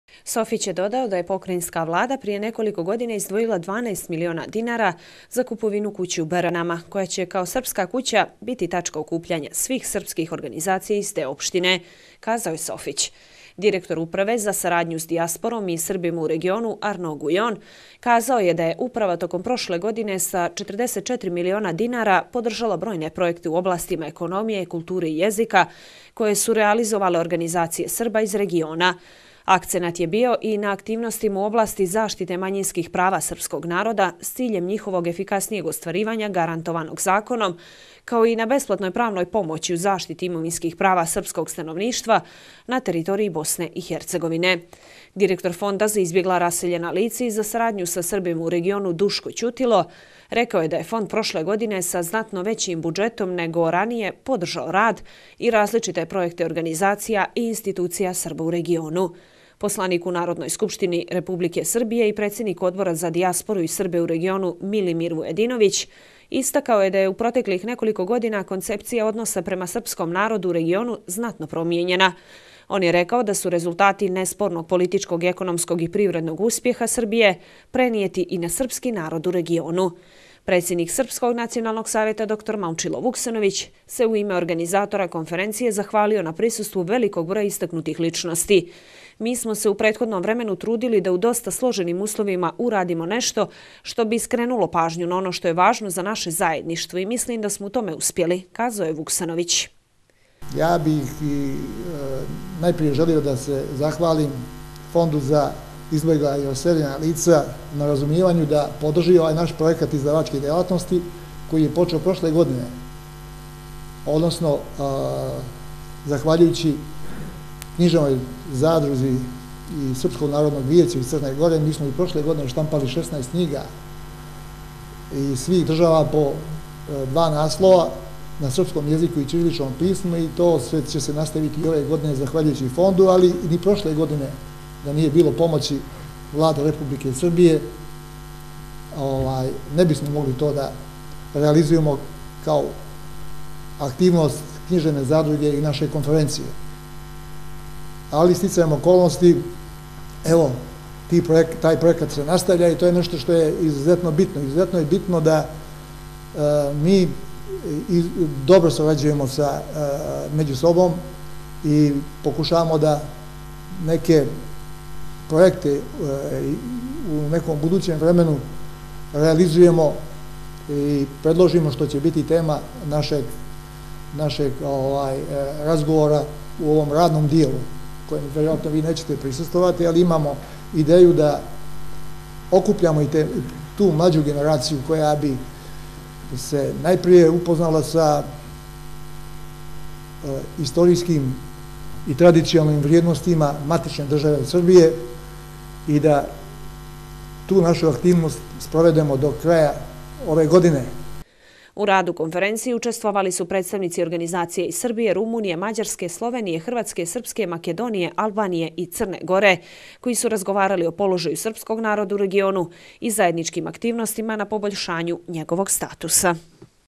Јединство представника српских институција и организација из региона резултат је стабилне Србије
Јединство представника српских институција и организација из региона резултат је стабилне и јаке Србије, која је препознала значај заједништва и коначно почела да се бави питањима Срба у региону“, казао је покрајински секретар за регионални развој, међурегионалну сарадњу и локалну самоуправу Александар Софић на отварању 13. Конференције српских националних организација из земаља региона одржане у Матици српској у Новом Саду.
Софић је додао да је  Покрајинска влада је пре неколико дана издвојила 12 милиона динара за куповину куће у Беранама, која ће, као Српска кућа, бити тачка окупљања свих српских организација из те општине“, казао је Софић.
Директор Управе за сарадњу с дијаспором и Србима у региону Арно Гујон казао је да је Упрва током прошле године са 44 милиона динара подржала бројне пројекте у областима економије, културе и језика, које су реализовале организације Срба из региона.
Акценат је био и на активностима у области заштите мањинских права српског народа, с циљем њиховог ефикаснијег остваривања гарантованог законом, као и на бесплатној правној помоћи у заштити имовинских права српског становништа на територији Босне и Херцеговине.
Директор Фонда за избегла, расељена лица и за сарадњу са Србима у регону Душко Ћутило рекао је да је Фонд прошле године са знатно већим буџетом него раније подржао рад и различите пројекте организација и институција Срба у региону.
Посланик у Народној скупштини Србије и председник Одбора за дијаспору и Србе у региону Милимир Вујадиновић истакао је да је у протеклих неколико година концепција односа према нашем народу у региону знатно промењена. Он је рекао да су резултати неспорног политичког, економског и привредног успеха Србије пренети и на српски народ у региону.
Предсједник Српског националног савјета Црне Горе др Момчило Вуксановић се у име организатора Конференције захвалио на присуству великог броја истакнутих личности.
"Ми смо се у претходном времену се трудили да у доста сложеним условима урадимо нешто што би скренуло пажњу на оно што је важно за наше заједништво, и мислим да смо у томе успјели," казао је Вуксановић.
У раду конференције учествовали су представници организација из Србије, Румуније, Мађарске, Словеније, Хрватске, Српске, Македоније, Албаније и Црне Горе, који су разговарали о положају српског народа у региону и заједничким активностима на побољшању његовог статуса.